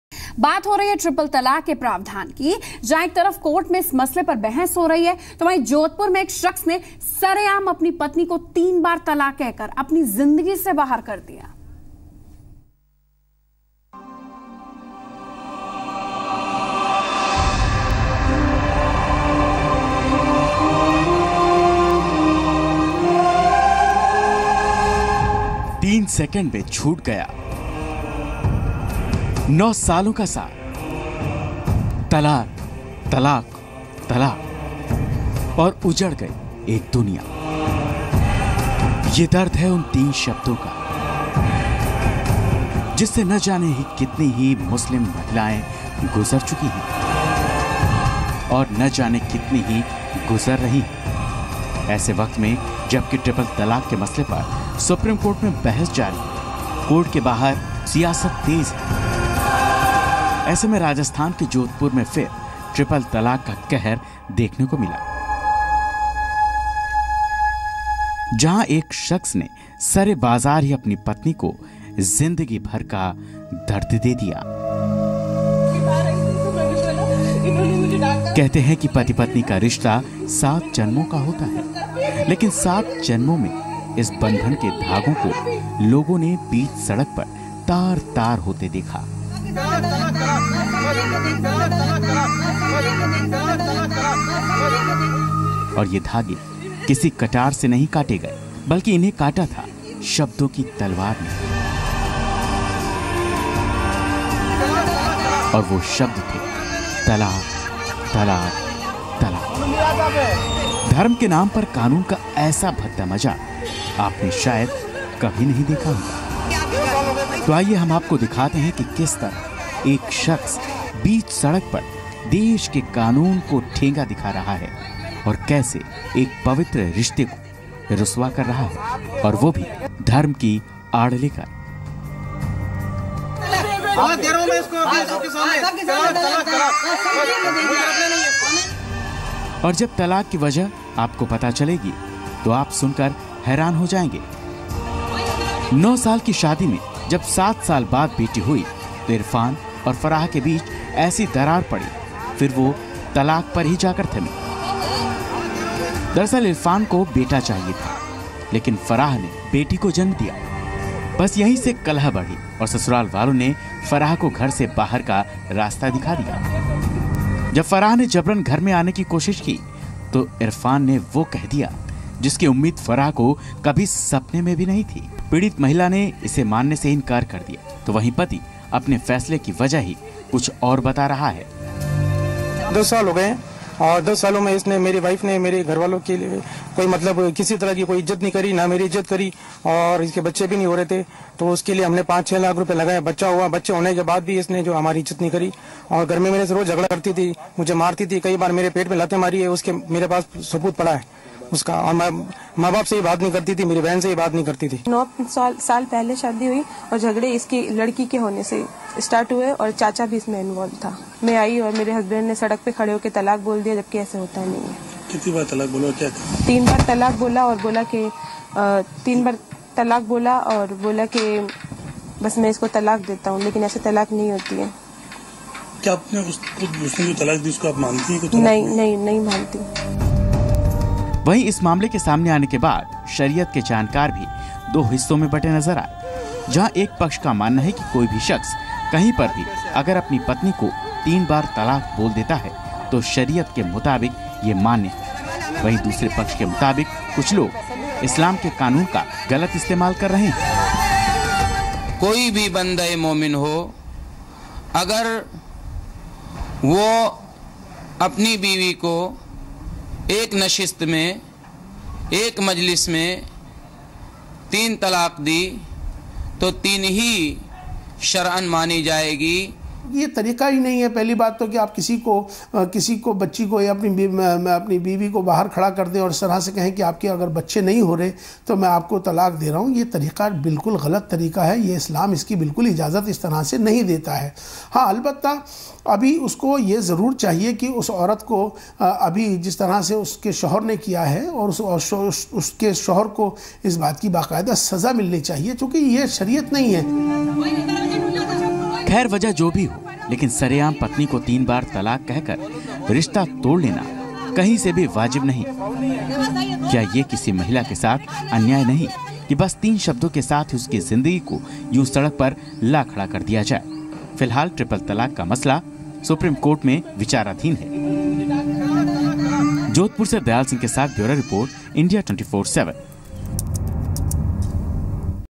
Jodhpur : A man gave divorce to his wife in public by using triple divorce system
A man gave divorce to his wife in public by using triple divorce system. Watch this special segment and get to know more here.